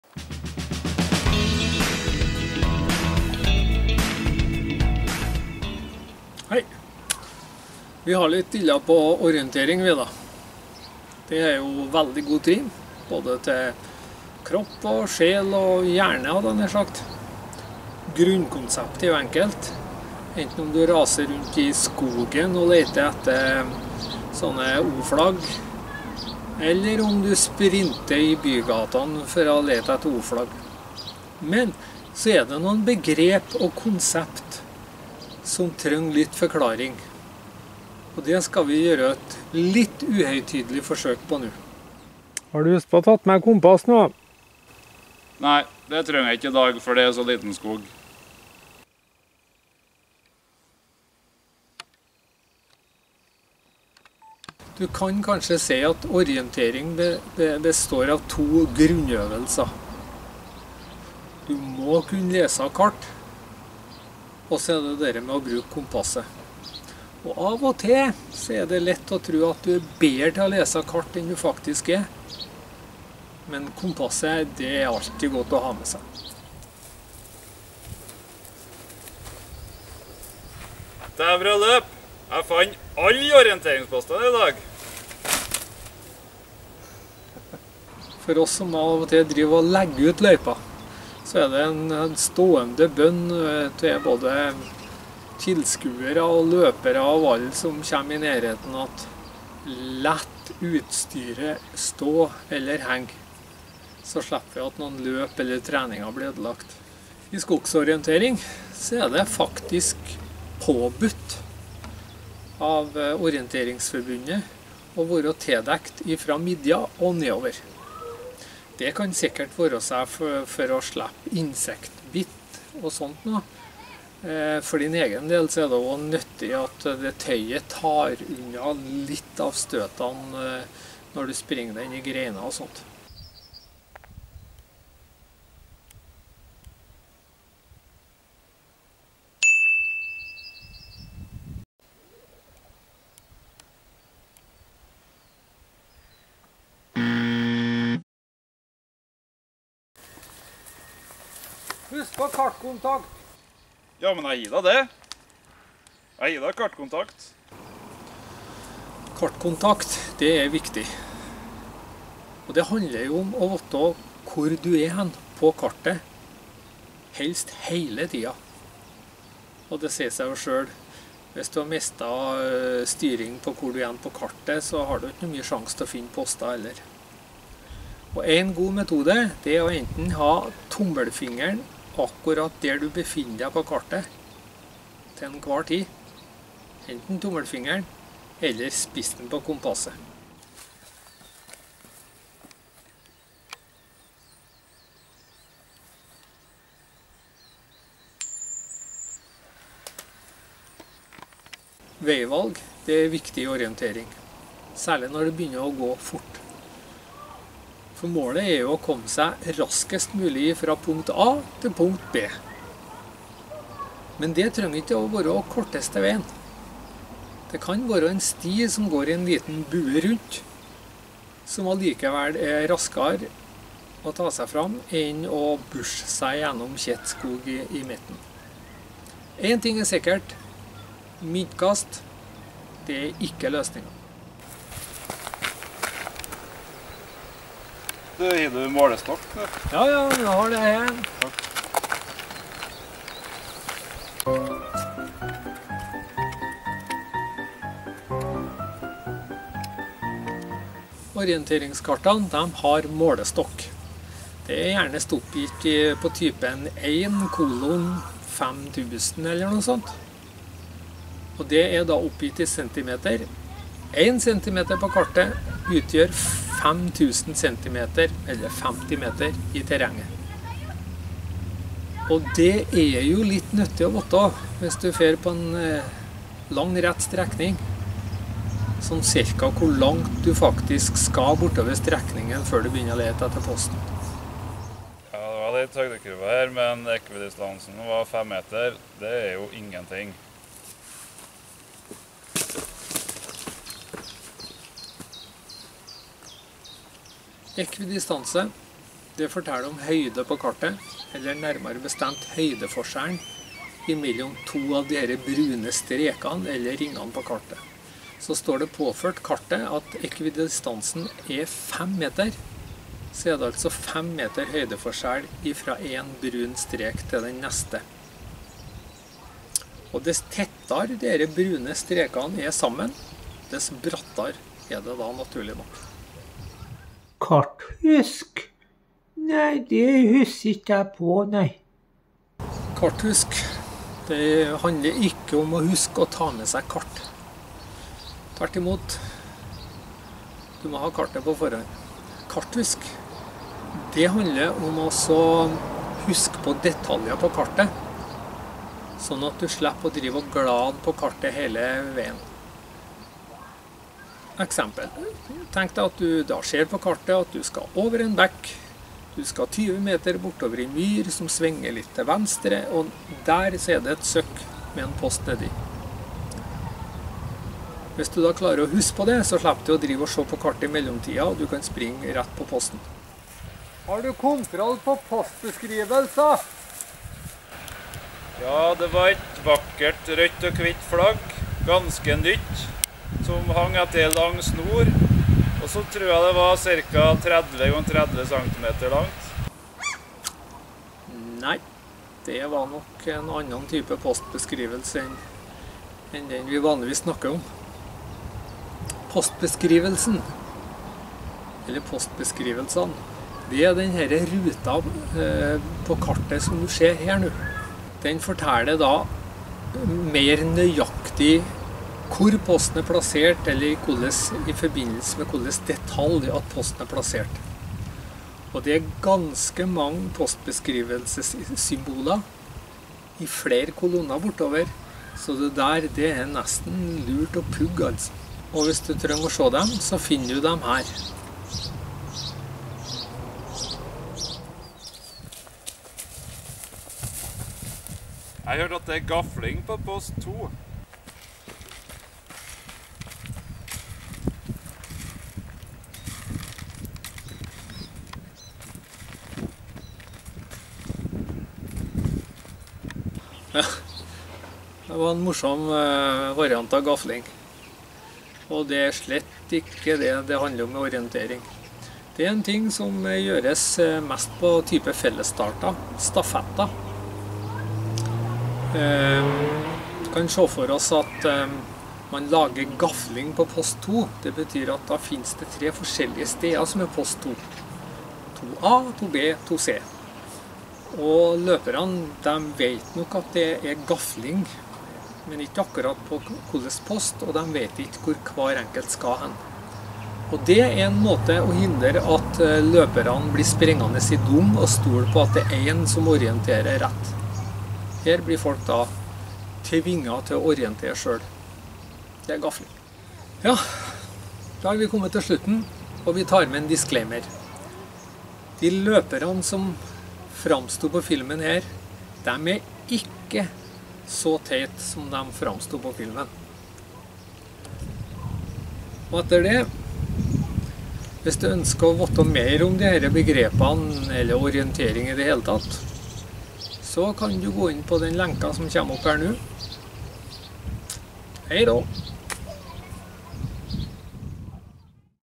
Orientering forklart
hofstadbros med Anton forsøker å redegjøre for begrep innenfor orientering.